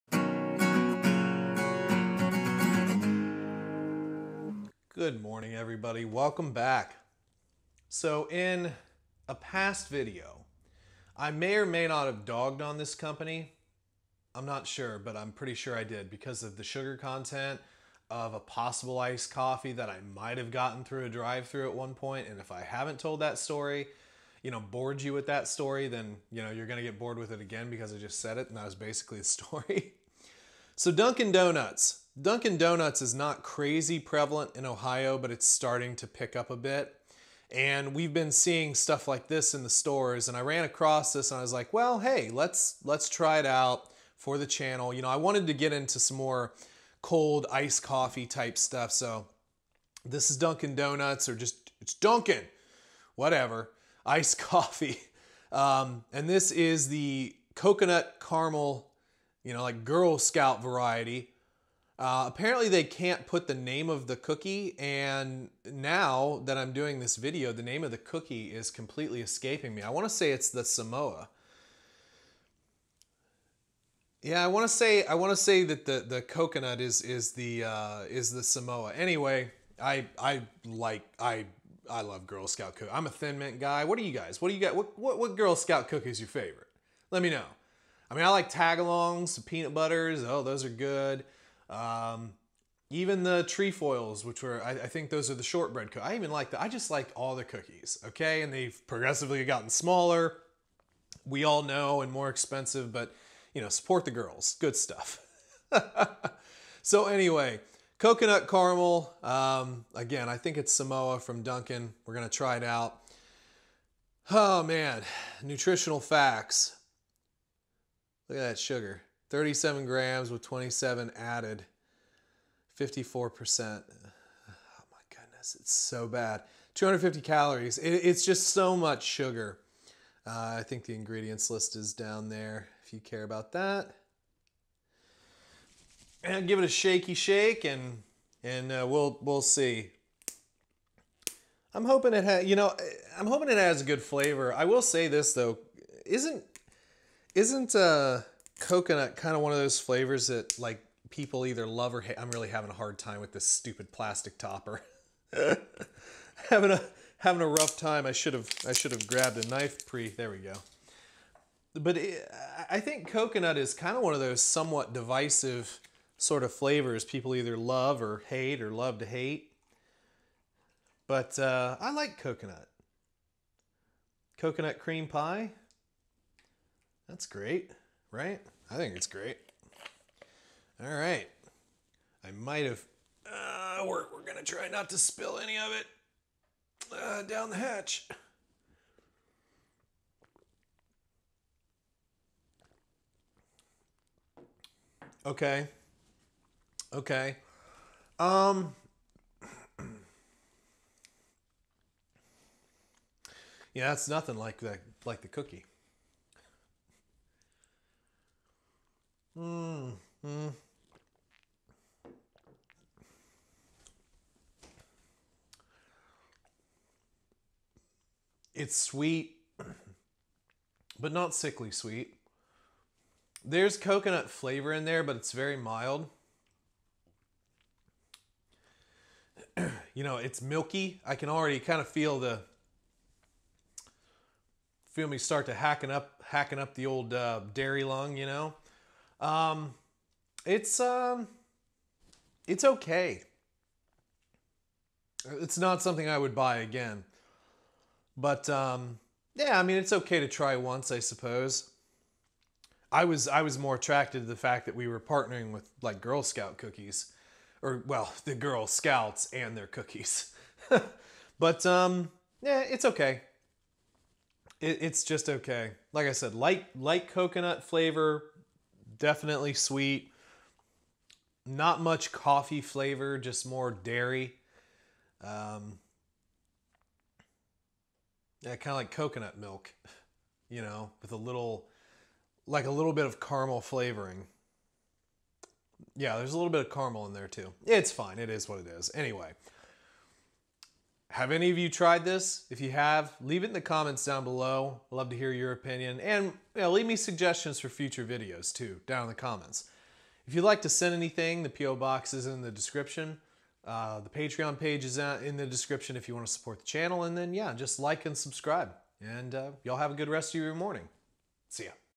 Monday Morning Coffee Break | Dunkin Donuts Iced Coffee | Girl Scout Coconut Caramel
Good morning everyone and welcome to your Monday morning coffee break! Bring your cup and your favorite brew as we explore different coffees every Monday morning.

Dunkin Donuts

Would you like to see something reviewed on the channel?

Mountain House
Wise Foods
Augason Farms